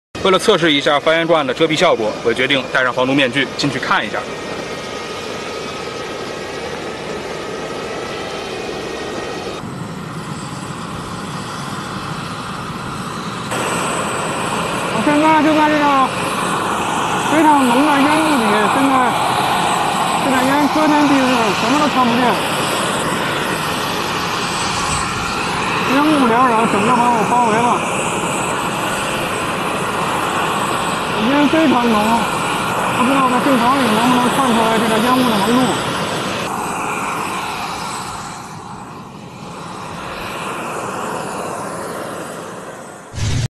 戴上防毒面具！军迷冲入烟雾内部一探究竟！
戴上防毒面具！军迷冲入烟雾内部一探究竟！（军迷行天下·解放军新闻传播中心融媒体出品）